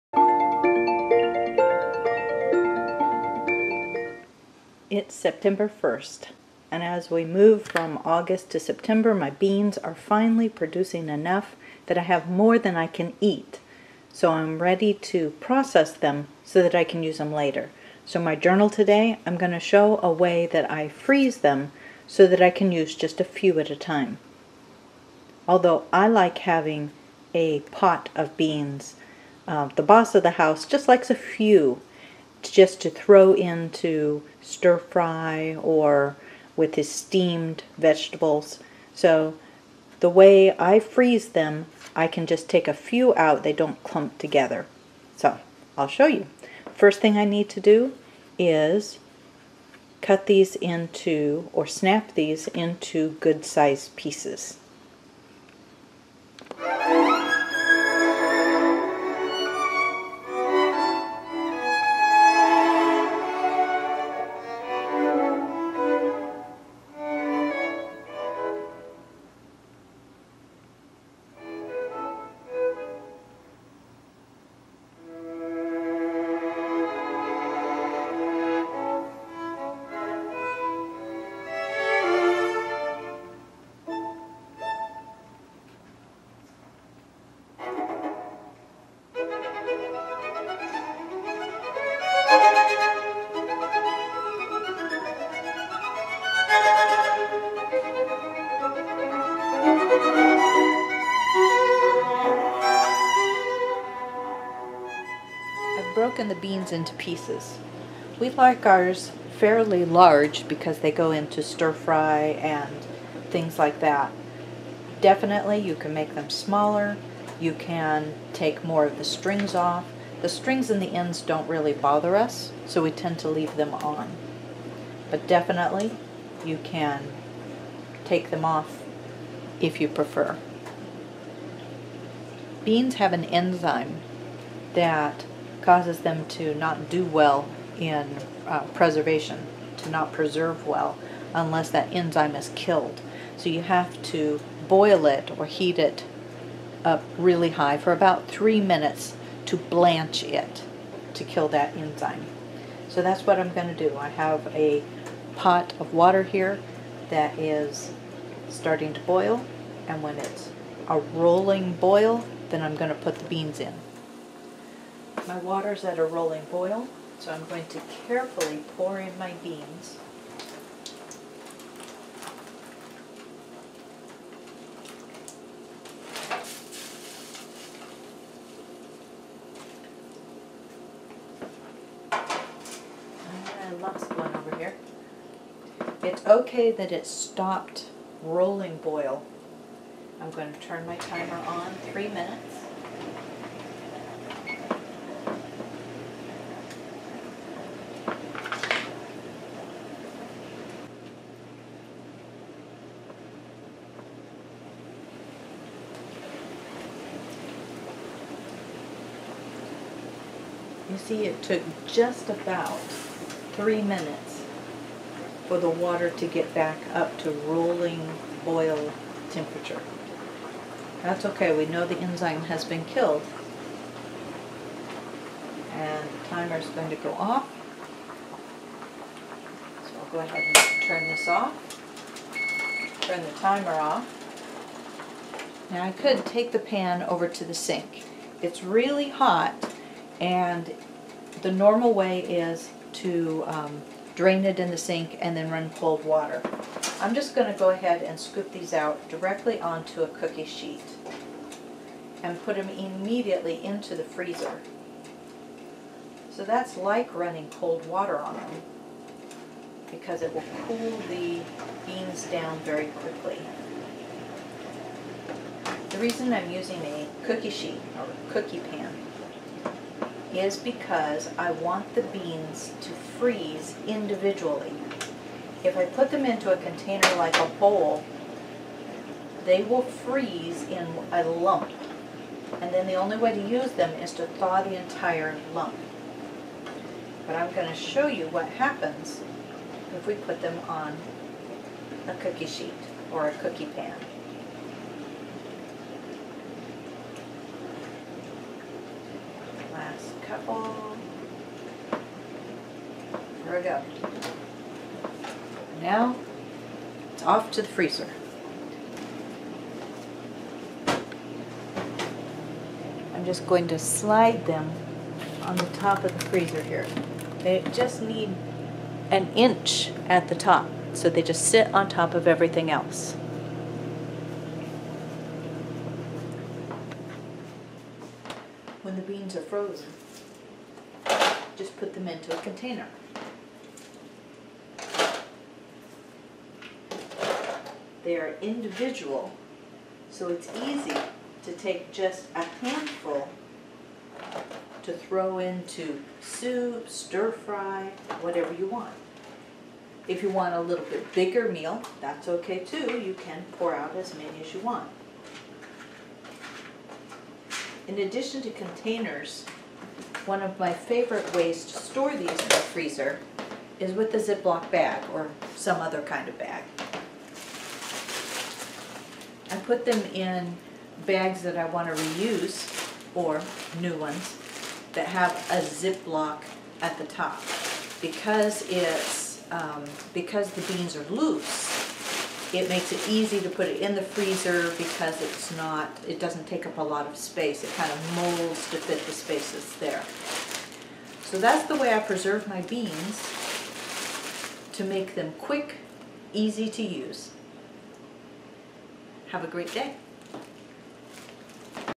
Freezing Green Beans for Quick Use Later-Migg's Urban Garden
Using this method of preserving beans, I can use them later a few at a time in soups, stir fry, or as a side dish. I freeze the beans individually after blanching.